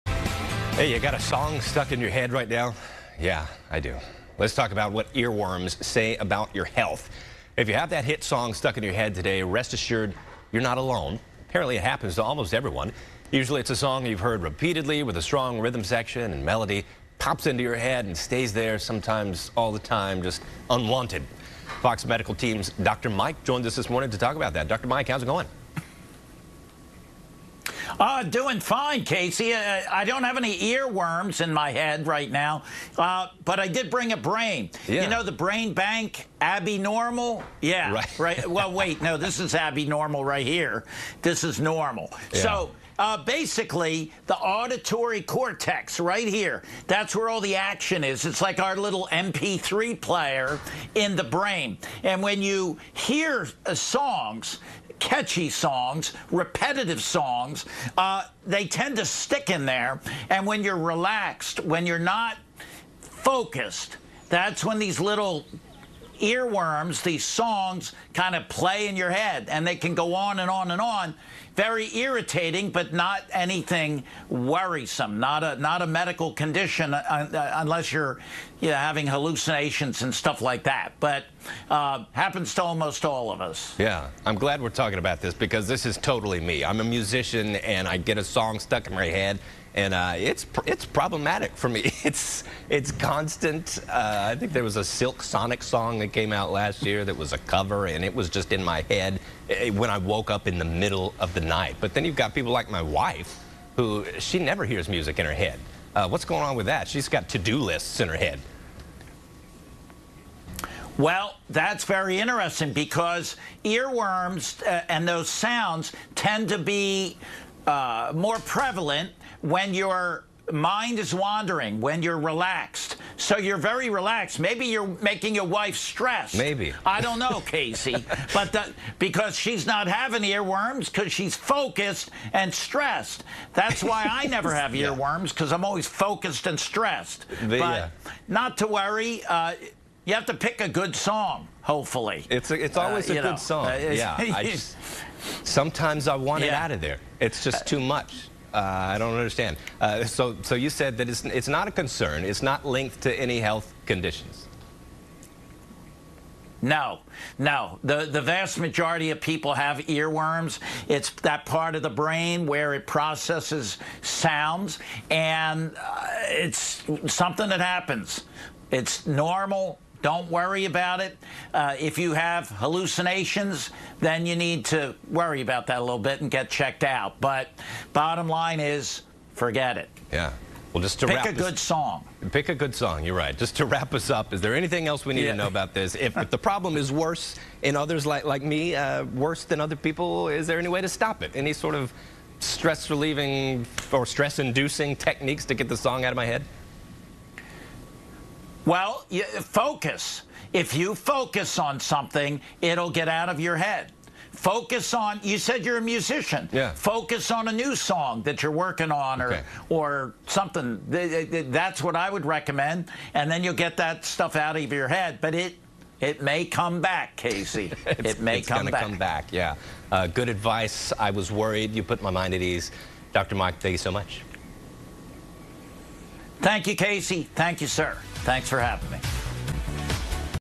Song stuck in your head? What it says about your health | FOX 7 Austin
We ask FOX Medical Team's Dr. Mike why music gets stuck in our heads and if there's any way to stop it.